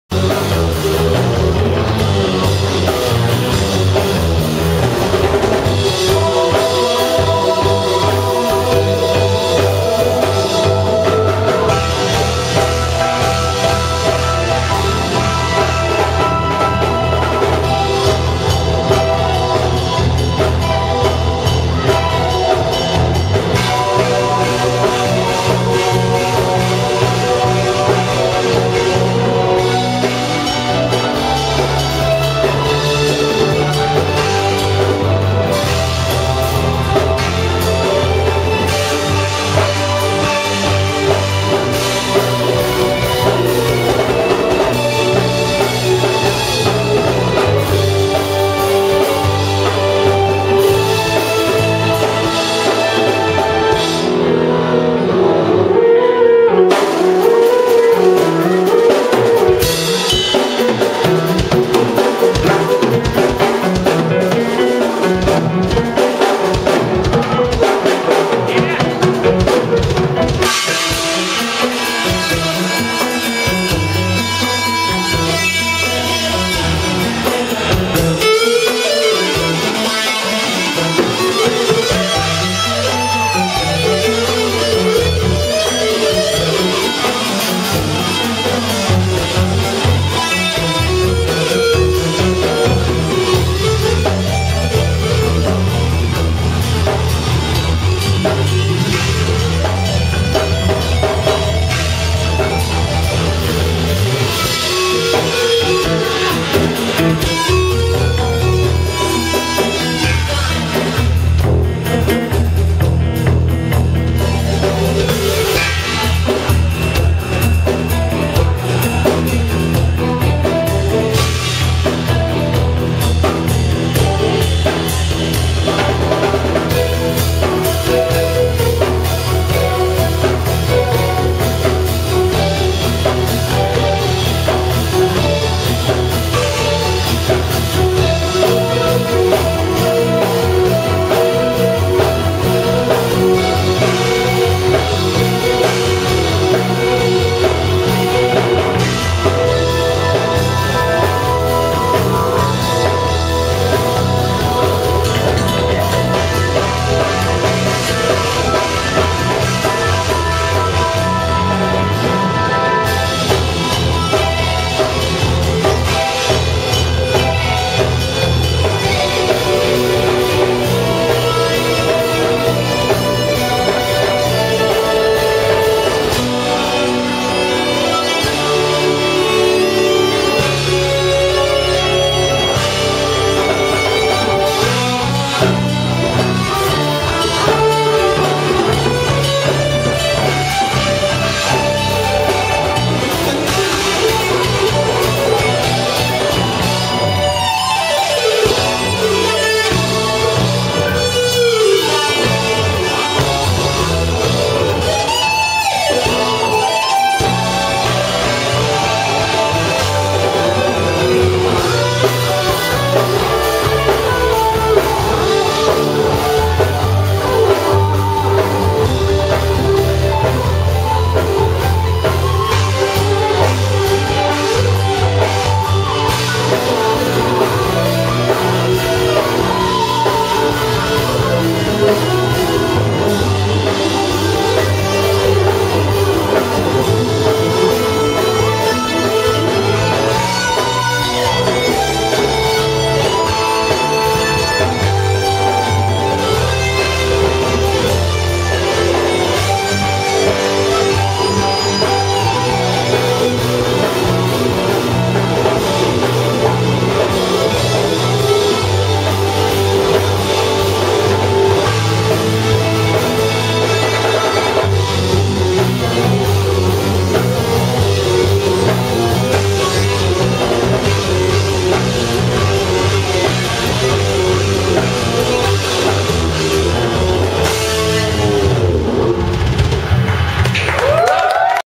Fuseboxx (Admit One 10th Anniv)
Fuseboxx performs at Admit One 10th Anniversary gig (raw video)

Freedom Bar, Anonas Quezon City
Philippines

Shot using Sanyo Xacti CG-10, in Stereo.